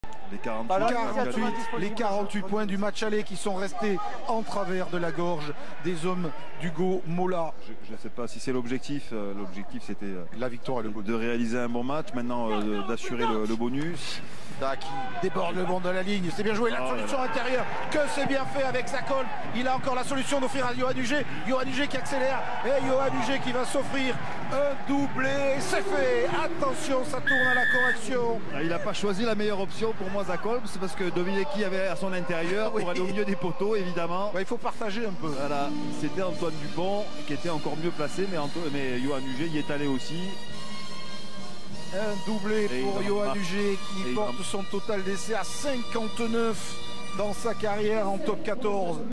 TOP 14 : Le deuxième essai de Yoann Huget a la toulousaine contre Paris
crédit canal +